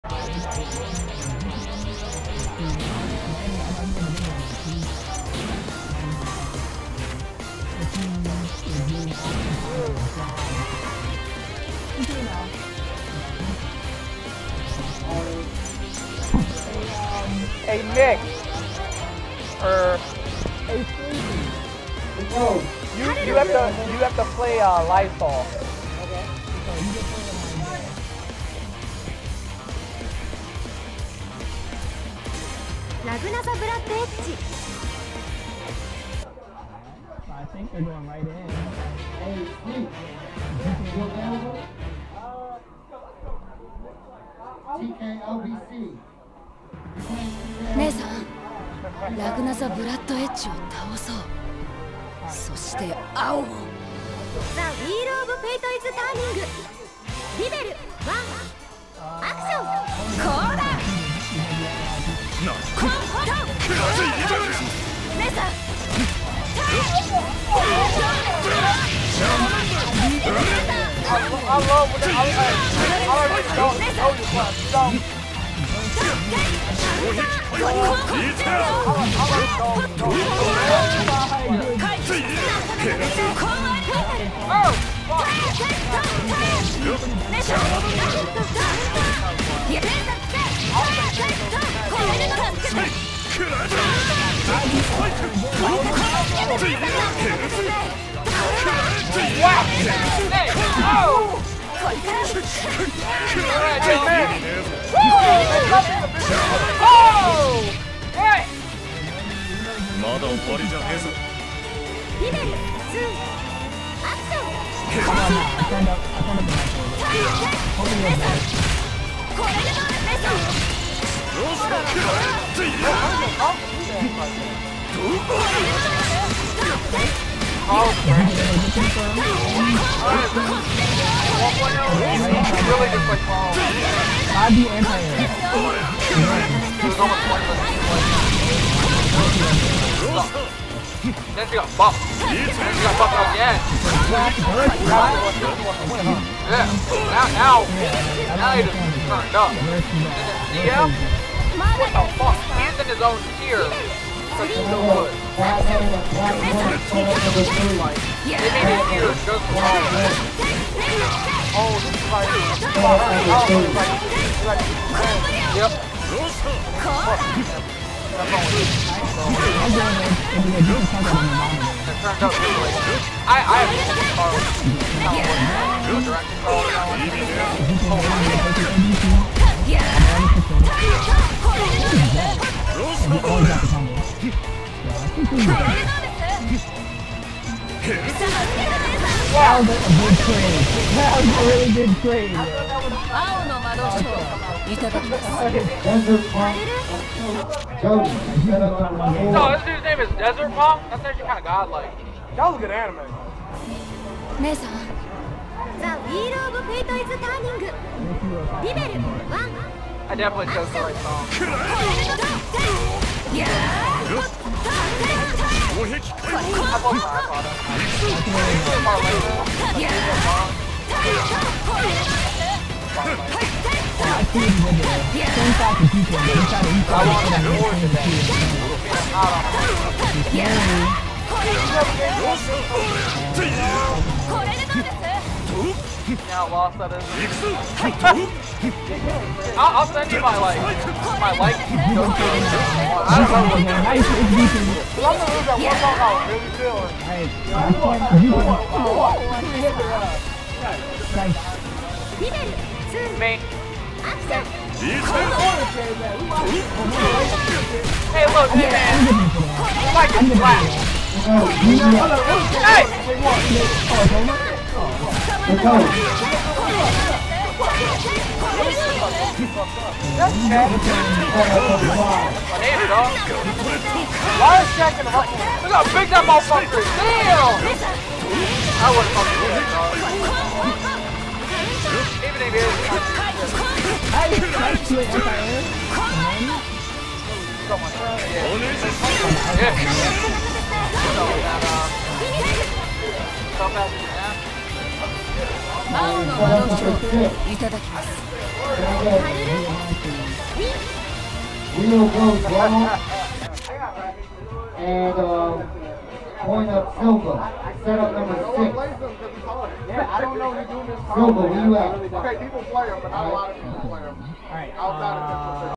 Waifu Wars 2.6 Manny vs NAH Jyuuken67
Winners Side Round 1

Manny - Ragna
NAH Jyuuken67 - Carl